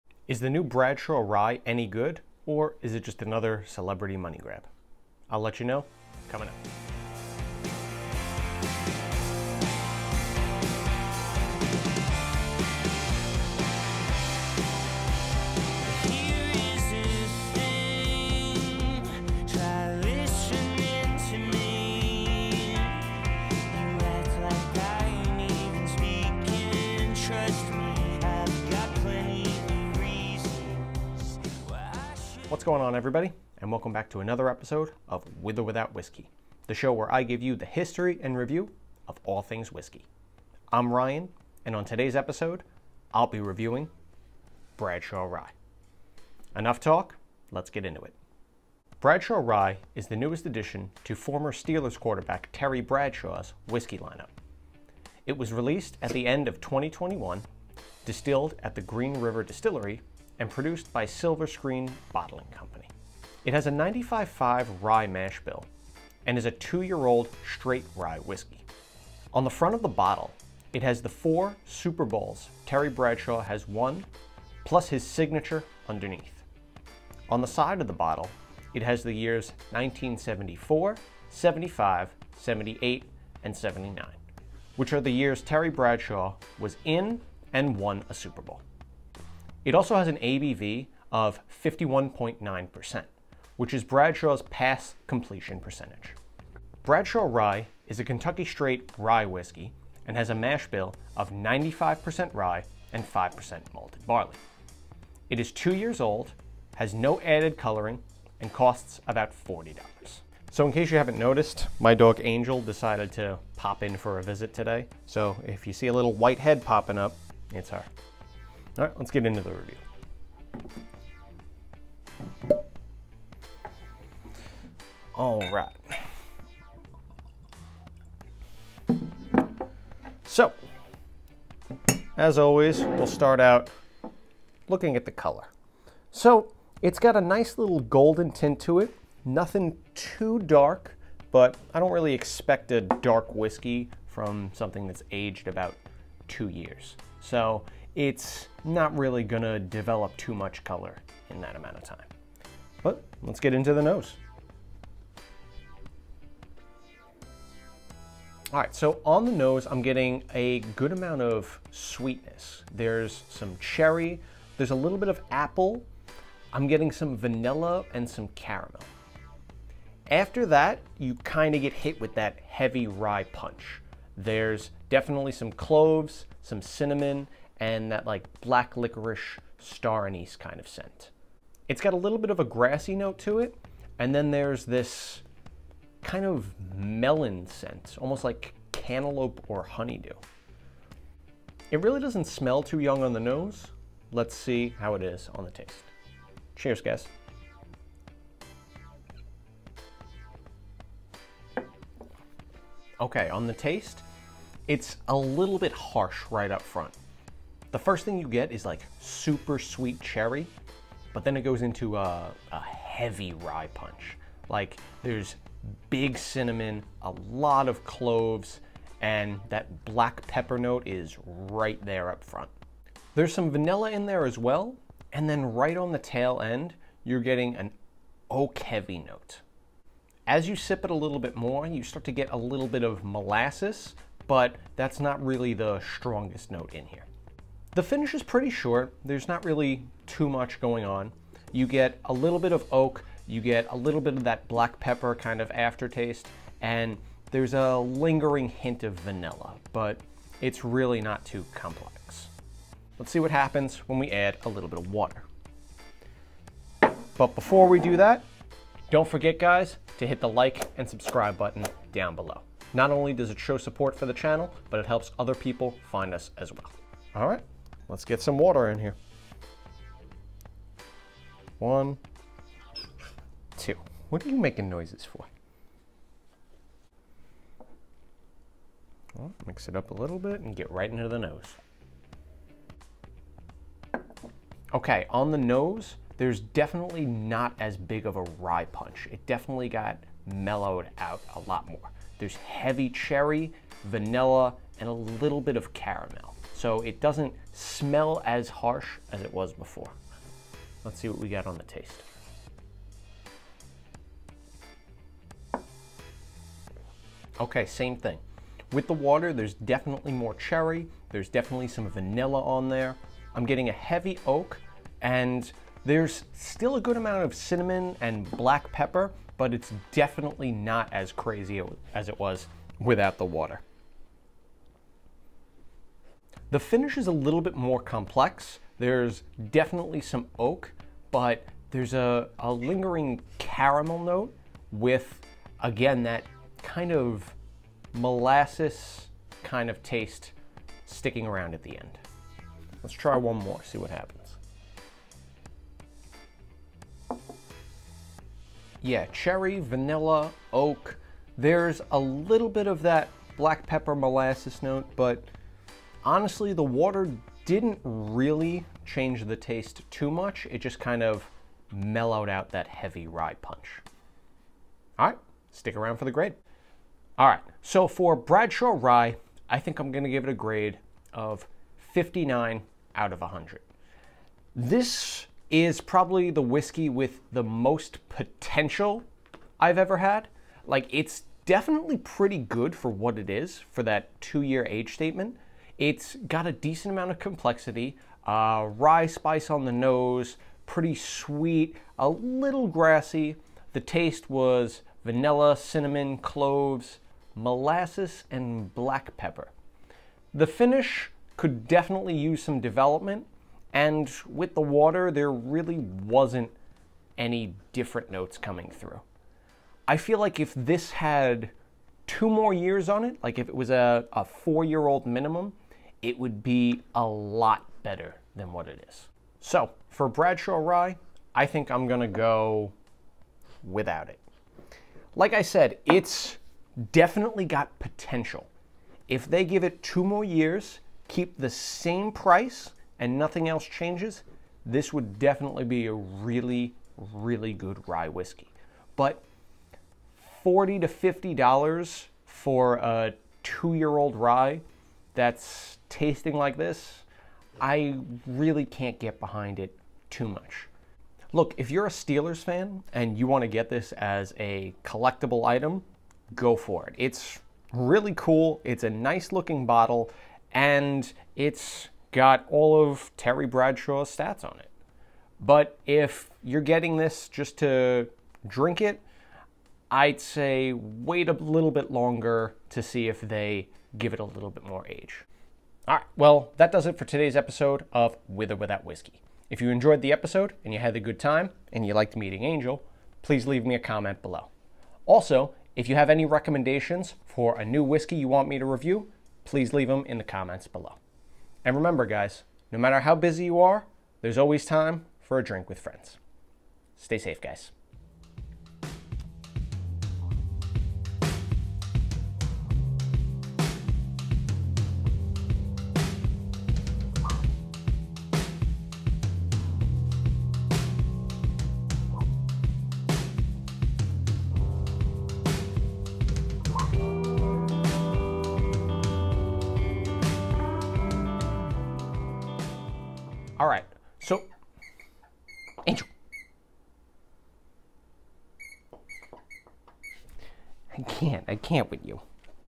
Bradshaw rye whiskey! Is it hall of fame worthy? EP# 20￼
Hey everybody on this week‘s episode of with the without whiskey I’m gonna be going over legendary Steelers quarterback Terry Bradshaw‘s new rye whiskey. ￼ Will it be as good as the man it’s named after or will it come up short?